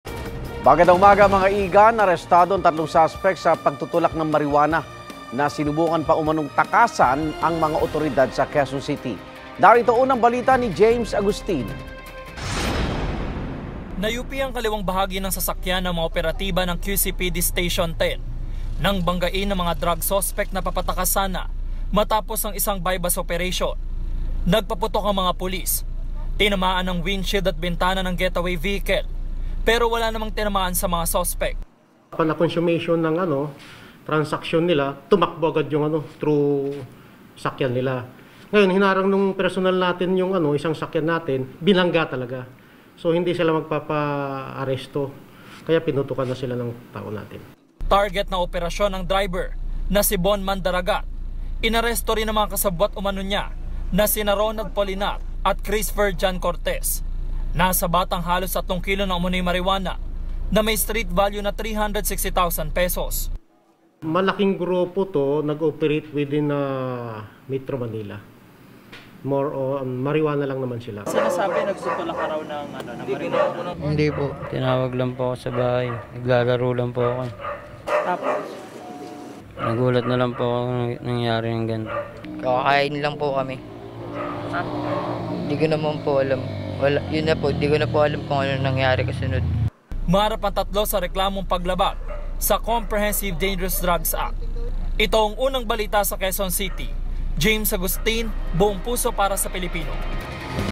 UB: 3 suspek sa pagtutulak ng droga, naaresto kahit tinangkang tumakas; P360,000 halaga ng...
UB: 3 suspek sa pagtutulak ng droga, naaresto kahit tinangkang tumakas; P360,000 halaga ng marijuana, nasabat